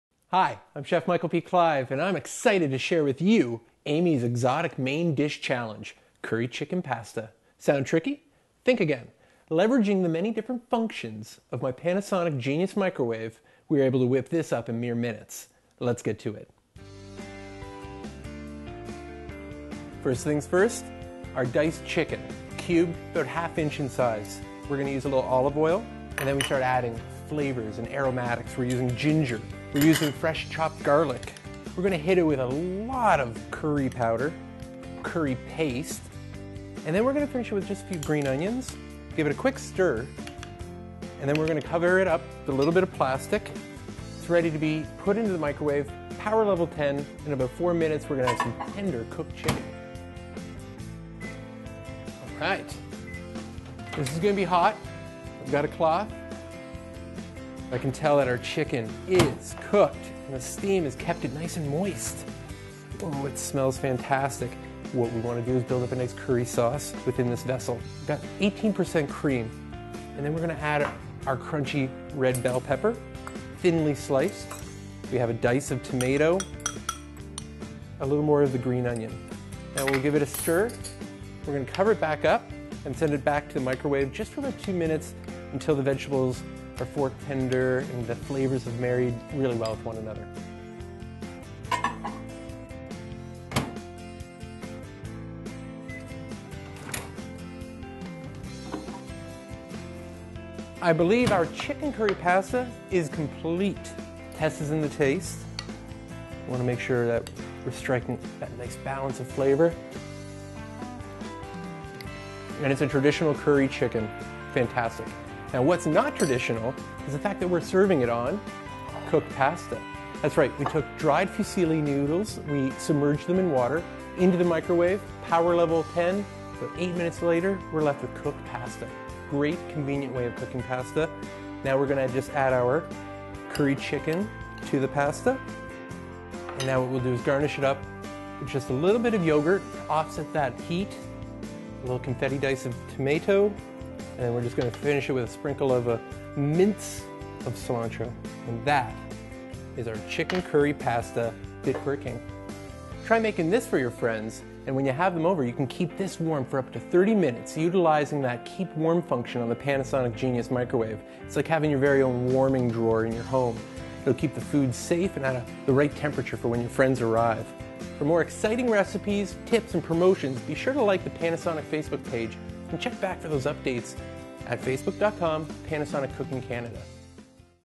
Curry Chicken Pasta - The Great Microwave Recipe Challenge
Chef Michael P. Clive tackles his third recipe challenge to create Curry Chicken Pasta in only a Panasonic Genius Inverter Microwave.